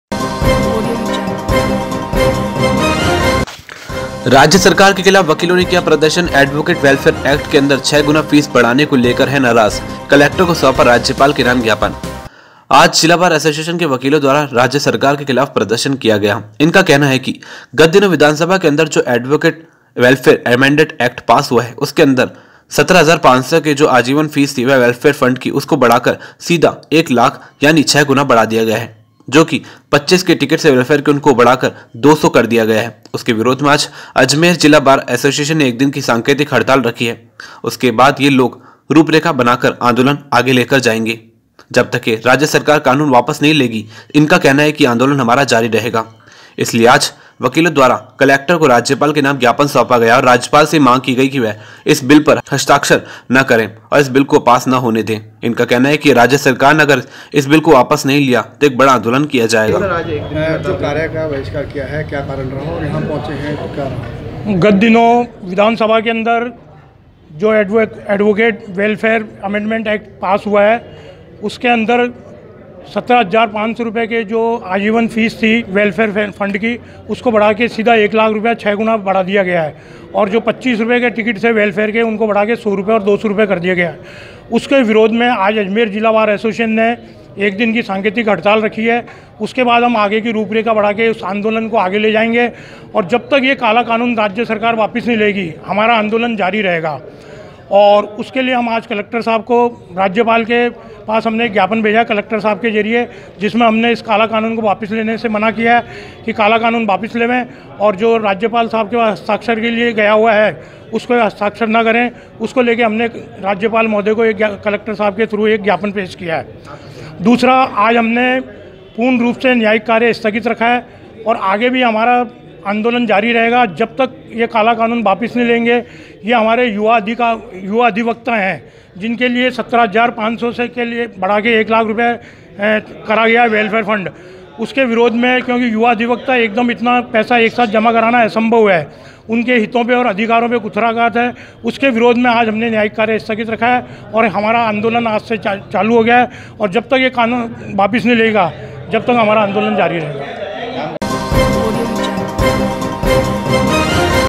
HORIZON HIND - राज्य सरकार के खिलाफ वकीलों ने किया प्रदर्शन
राज्य सरकार के खिलाफ वकीलों ने किया प्रदर्शन